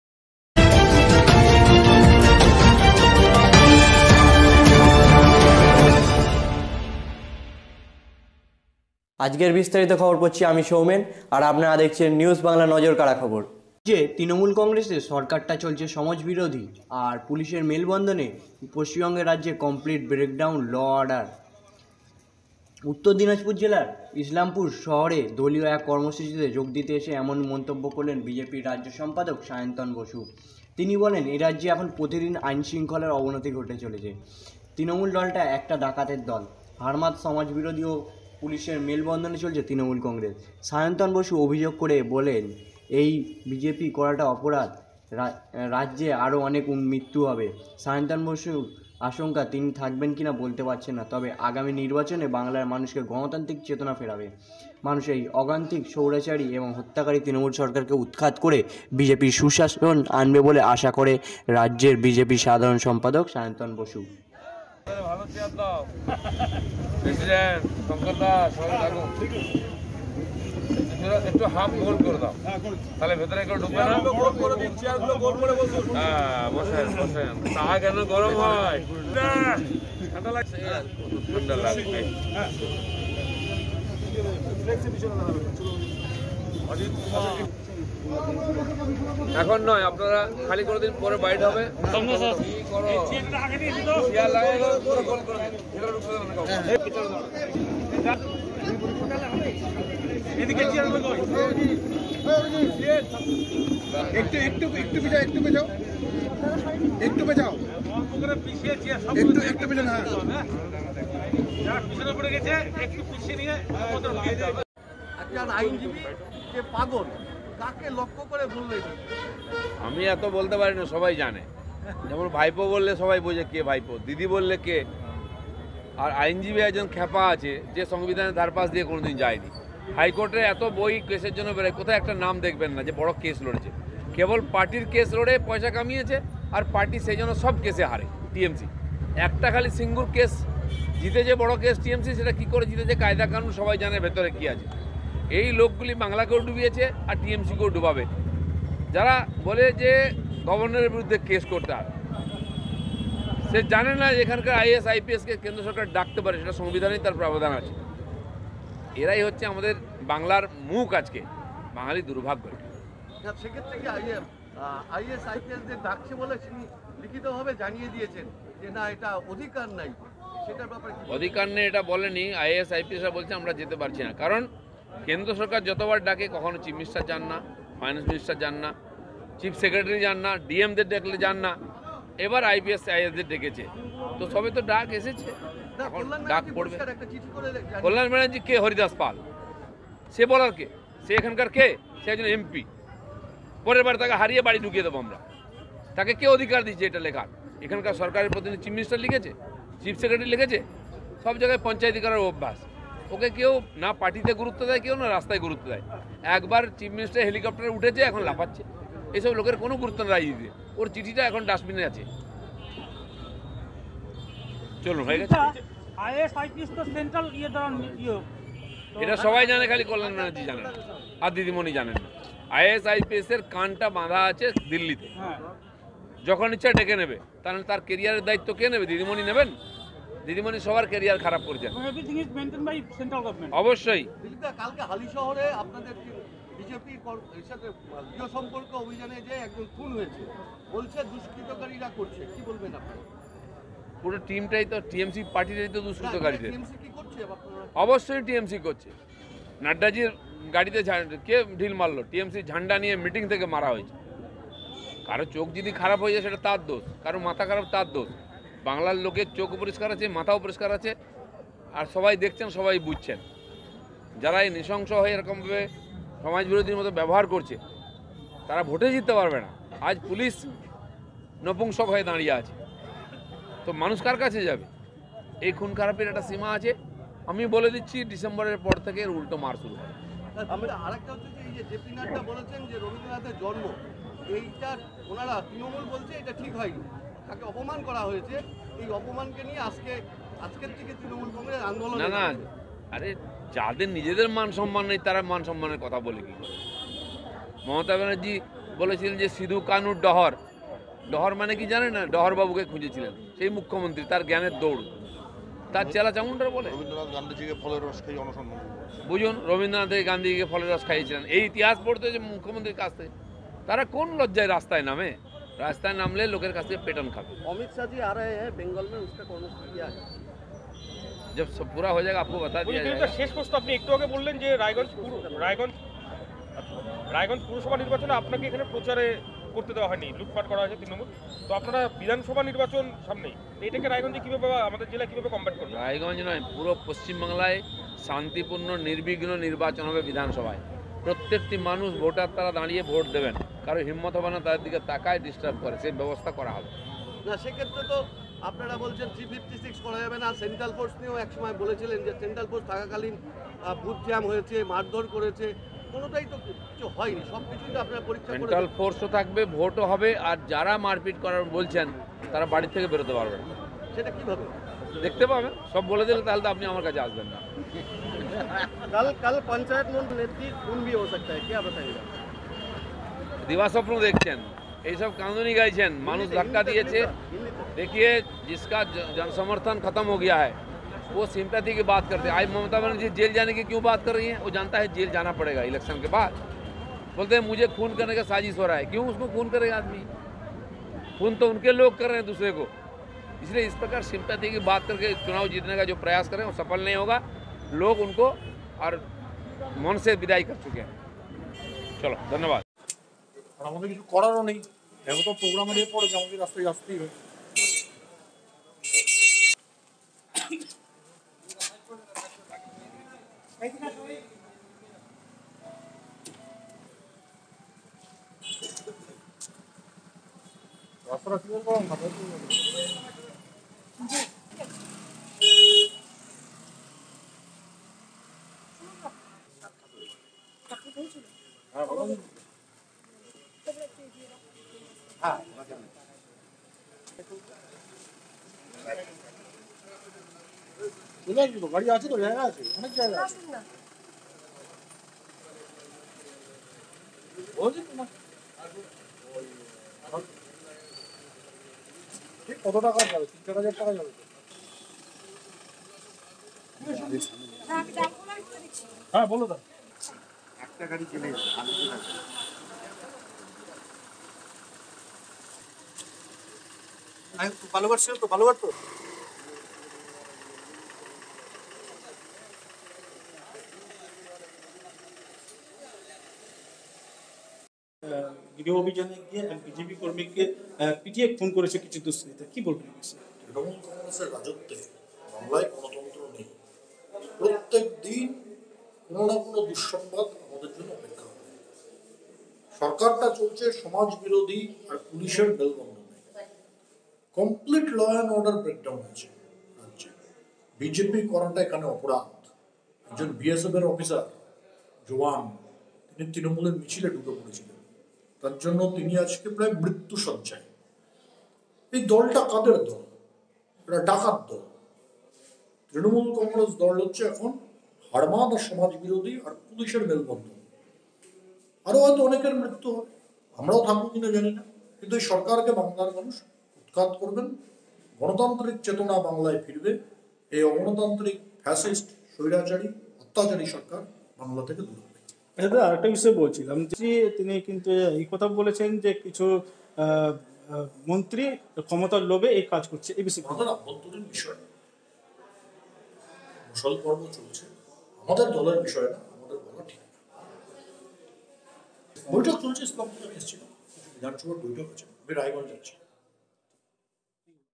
13 December 2020
" রাজ্যে তৃনমূল কংগ্রেসের সরকারটা চলছে সমাজবিরোধী আর পুলিশের মেলবন্ধনে, পশ্চিমবঙ্গ রাজ্যে কমপ্লিট ব্রেকডাউন ল অ্যান্ড অর্ডার "।  উত্তর দিনাজপুর জেলার ইসলামপুর শহরে দলীয় এক কর্মসূচিতে যোগ দিতে এসে এমন মন্তব্য করলেন বিজেপির রাজ্য সাধারণ সম্পাদক সায়ন্তন বসু।  তিনি বলেন এরাজ্যে এখন প্রতিদিন আইনশৃঙ্খলার অবনতি ঘটে চলেছে।  তৃনমূল দলটা একটা ডাকাতের দল। হার্মাদ সমাজবিরোধী ও পুলিশের মেলবন্ধনে চলছে তৃণমূল কংগ্রেস।  সায়ন্তন বসু অভিযোগ করে বলেন এই রাজ্যে বিজেপি করাটা অপরাধ। রাজ্যে আরও অনেক মৃত্যু হবে। সায়ন্তন বসুর আশঙ্কা তিনিও থাকবেন কিনা বলতে পারছেন না। তবে আগামী নির্বাচনে বাংলার মানুষের গনতান্ত্রিক চেতনা ফিরবে। মানুষ এই অগনতান্ত্রিক স্বৈরাচারী এবং হত্যাকারী তৃনমূল সরকারকে উৎখাত করে বিজেপির সুশাসন আনবে বলে আশা করে রাজ্য বিজেপির সাধারণ সম্পাদক সায়ন্তন বসু।